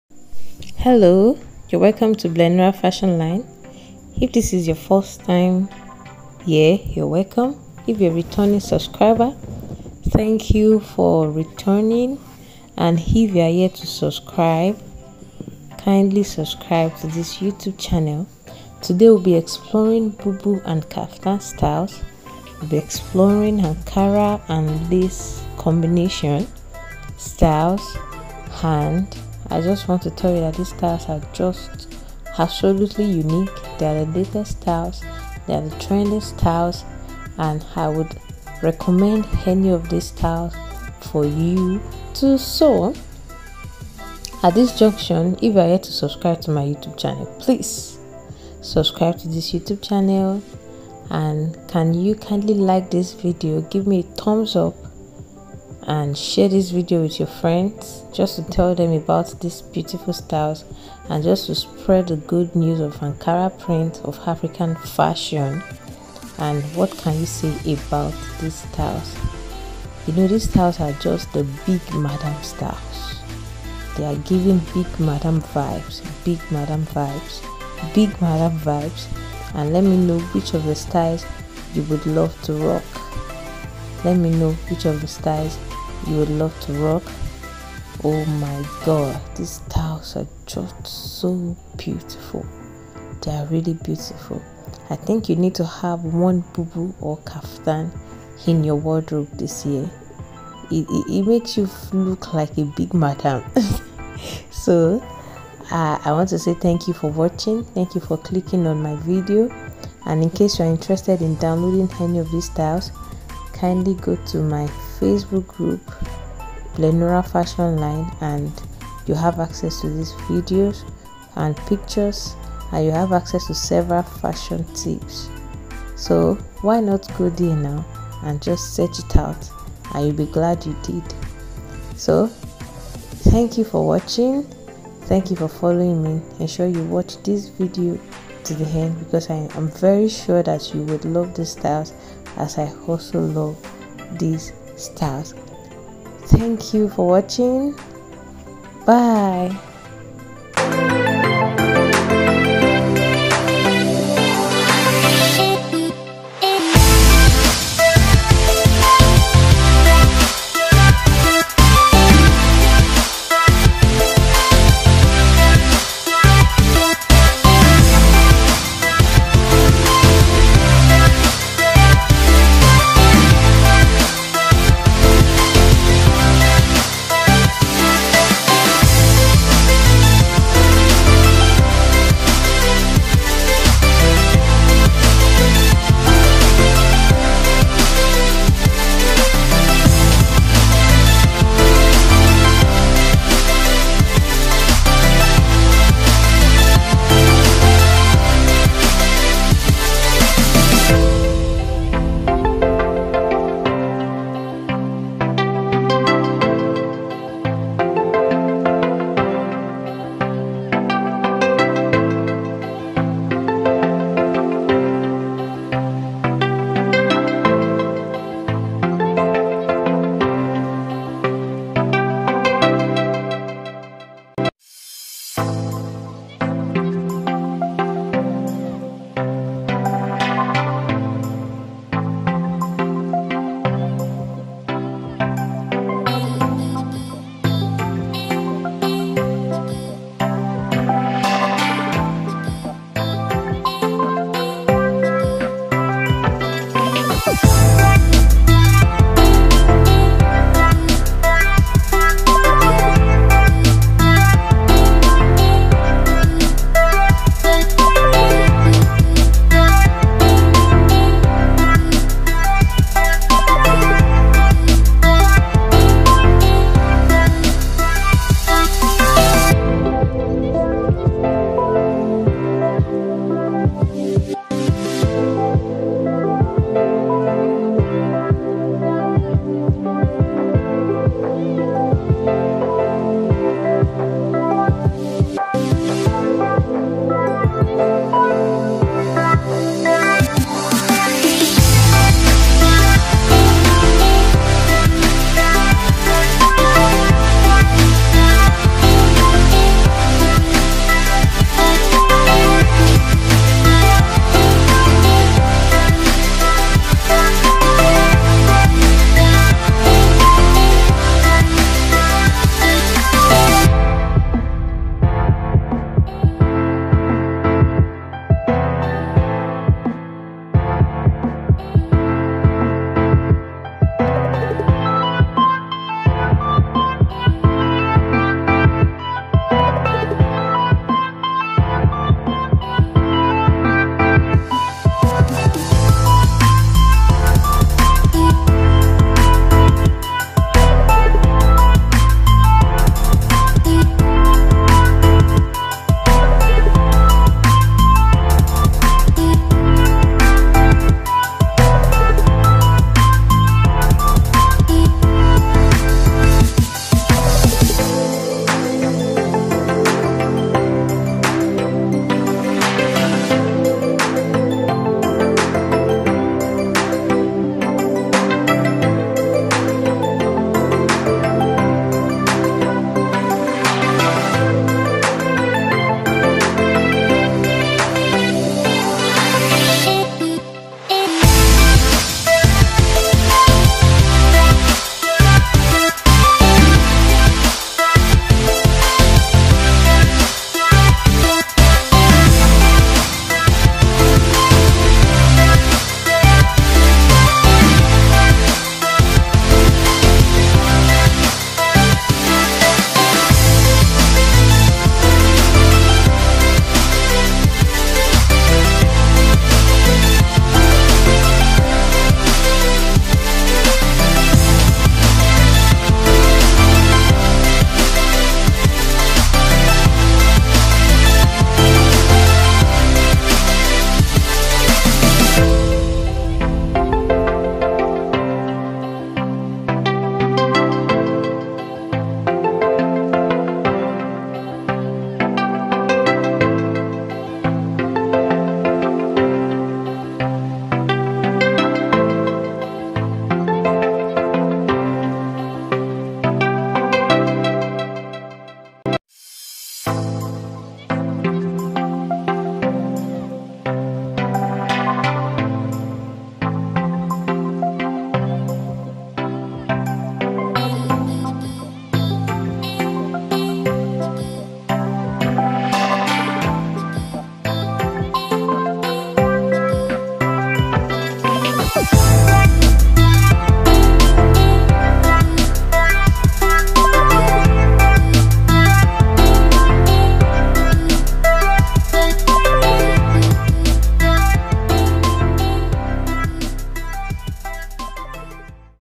Ankara & Lace Combination Styles | Most Beautiful and Unique Boubou and Kaftan Styles 2023
Hello lovers of African Fashion:

In this video, I bring to you the latest Ankara and Lace Gown Styles for Ladies 2023.

These styles are unique, sophisticated and unique Ankara + Lace Styles you can rock to a Party.

African Fashion Styles, Lace Styles, modele en pagne, Ankara Styles 2023.

MUSIC

All the credit in this video goes to all the amazing and talented designers that designed these outfits.